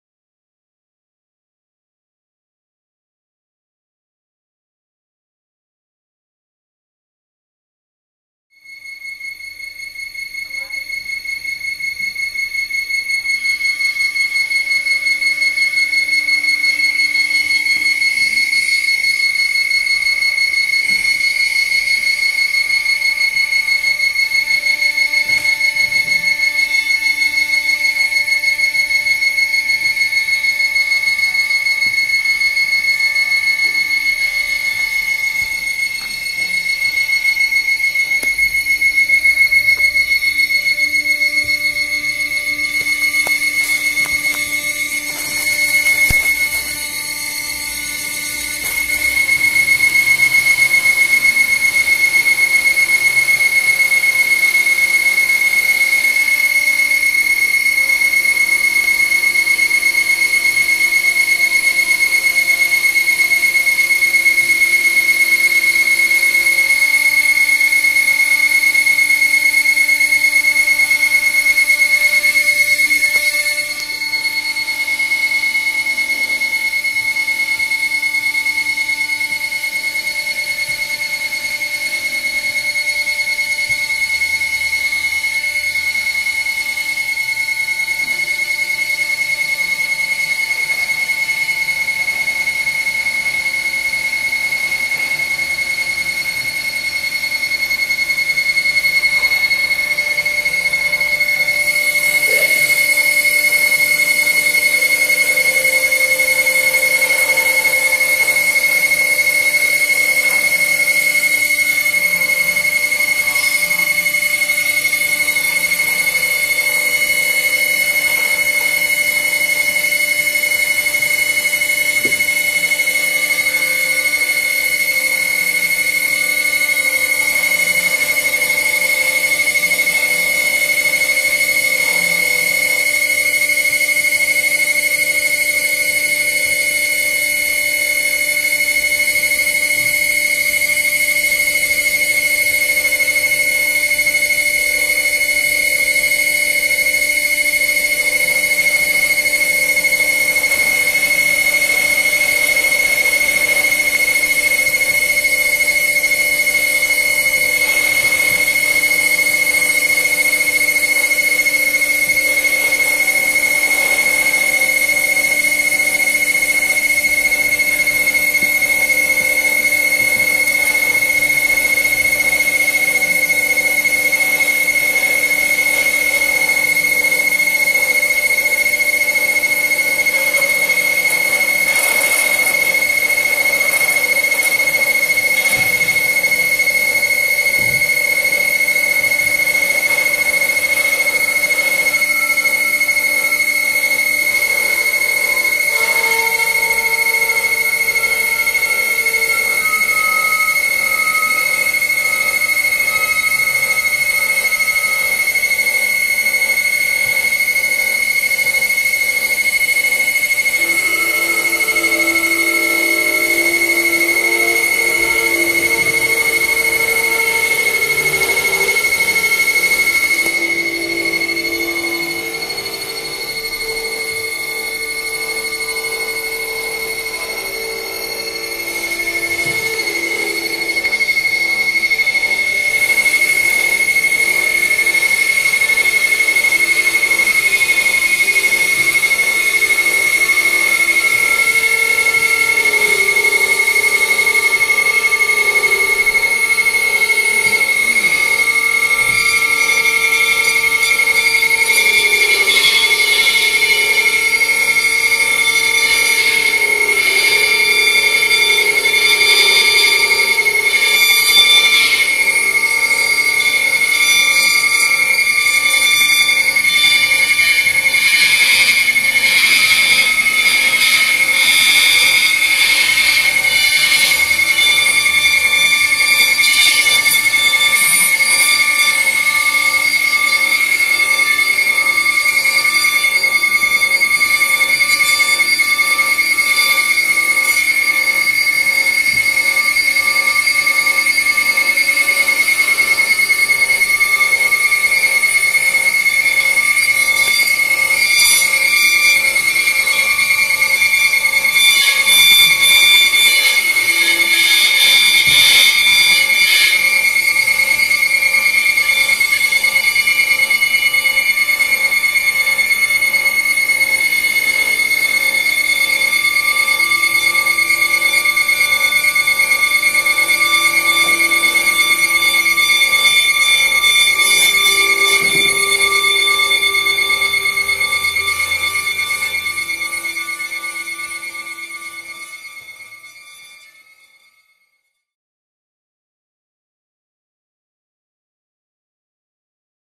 Tin Tabernacle Performance, Video by Bill Thompson aka prof_lofi
This is a lof-fi excerpt of my performance at the recent Night of the Tin Tabernacle event at The Tin Tabernacle in London.

I used found objects from the space to perform with using hand-held transducers.